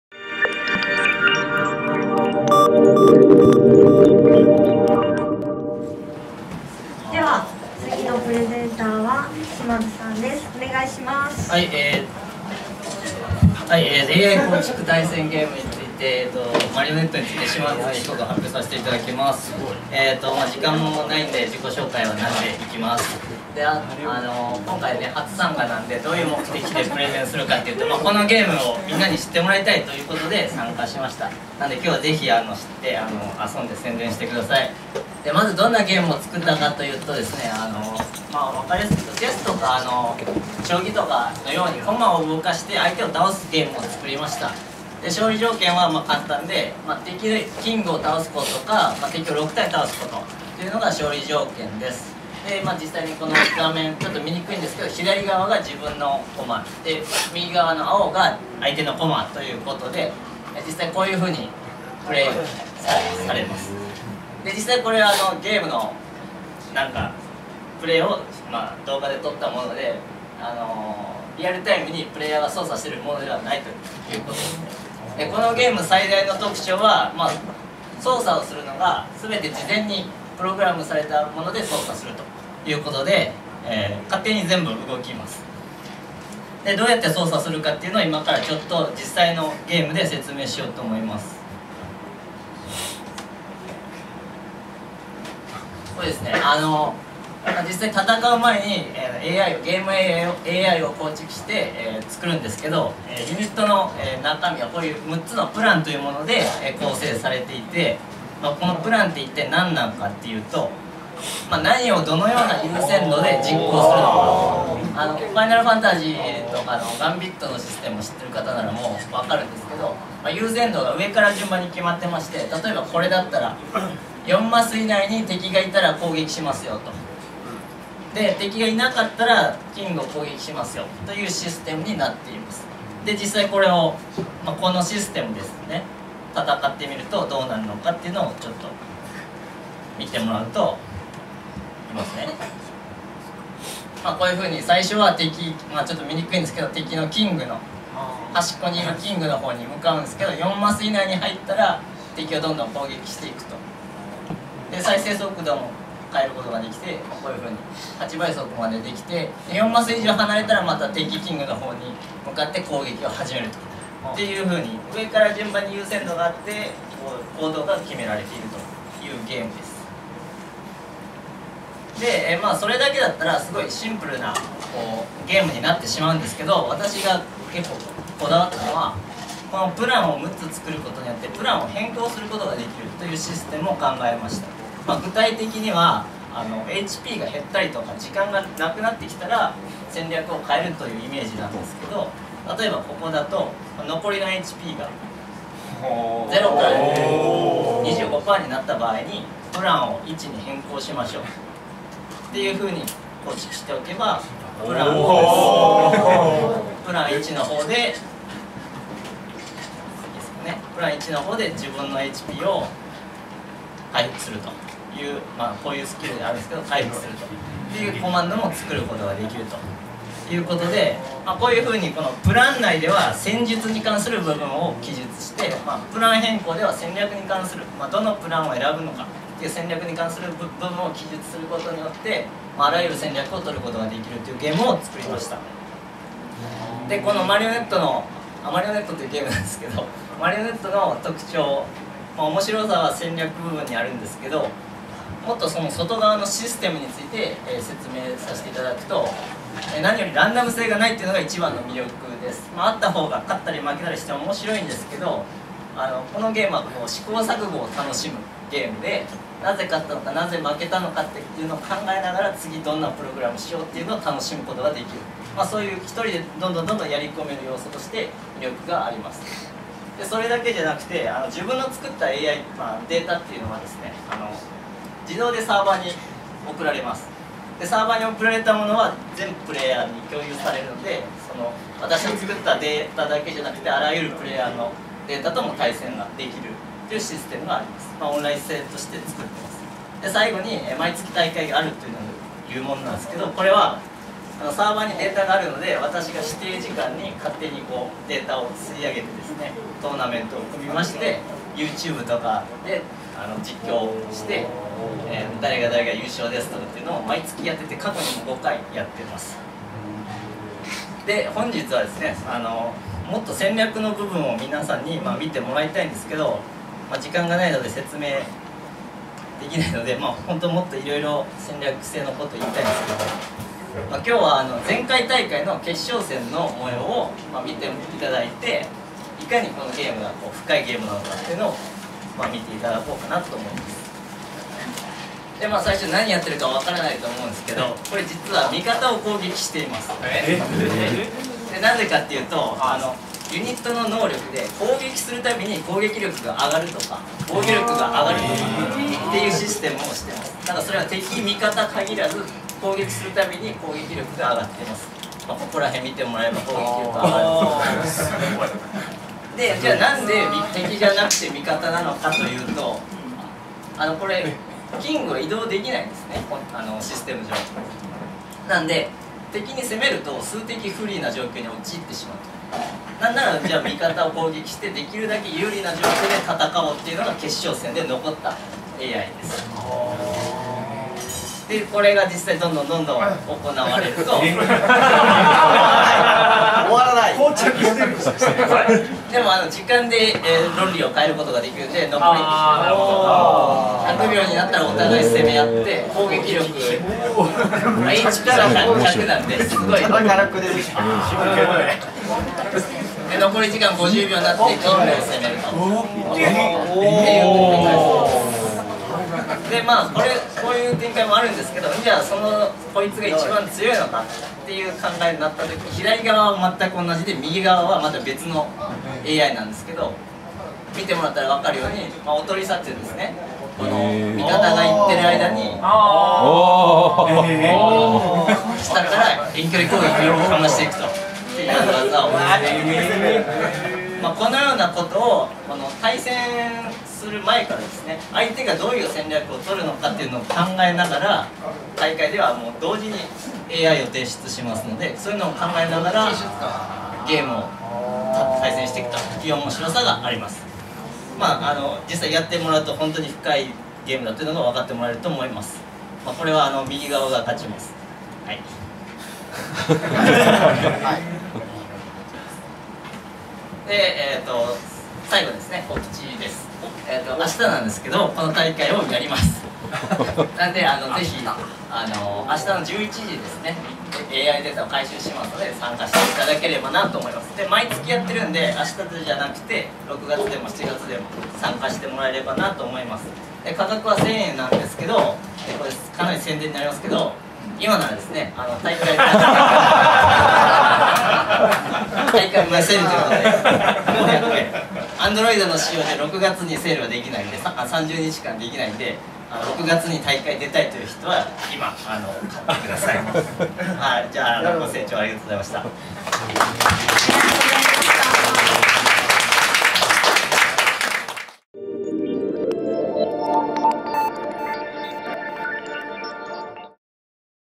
【ichi pixel】20170526_「AI構築型対戦ゲーム マリオネットについて」
「AI構築型対戦ゲーム マリオネット」

チェスや将棋の様に直接指示を出すのではなく、
駒に対してAIの指示を与えながら、敵を倒していく「マリオネット」を作られた嶋津恒彦さん。

AIを組む様子や、対戦の模様を語って頂きました。

●Edit by Hiroshi Ogawa
-Back to the BASIC- 808